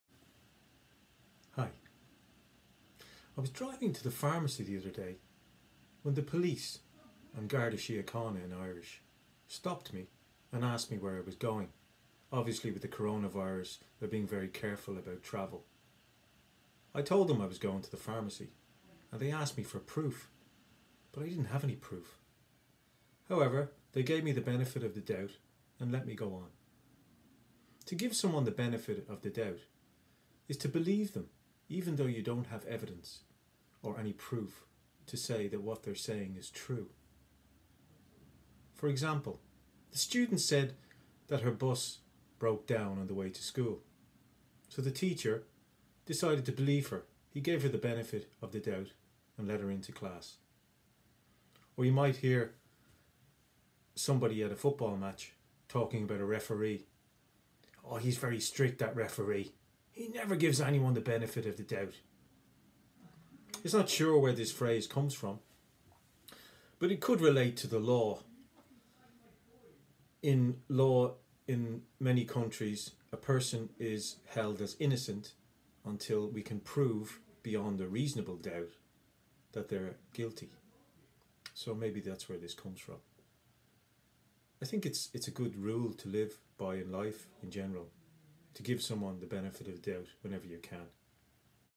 The Benefit of the Doubt
Our Director of Studies, David, explains the meaning of the phrase 'the benefit of the doubt.' This video is a part of an online series of explanations of common English phrases.